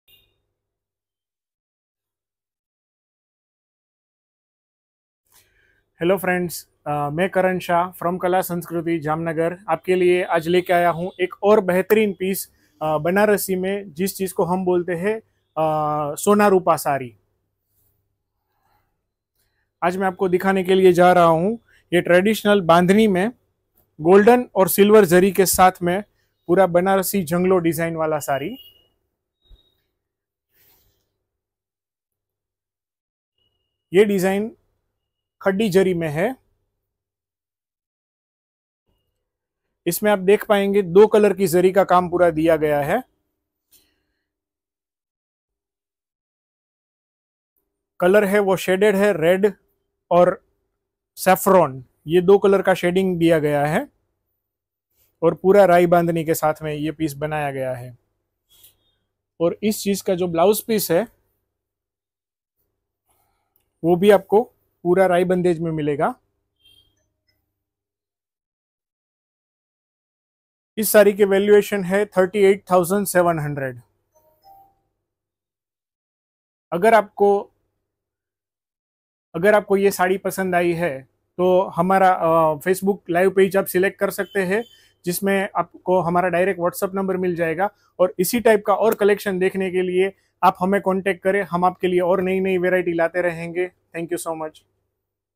KSRPL Red & Safran Sona Roopa Saree - Banarasi Elegance
This exquisite Sona Rupa saree is handwoven in the traditional Banarasi style with a stunning red & saffron colour shading. Intricate zari work and floral motifs adorn the saree's design. The perfect blend of elegance and tradition! 🇮🇳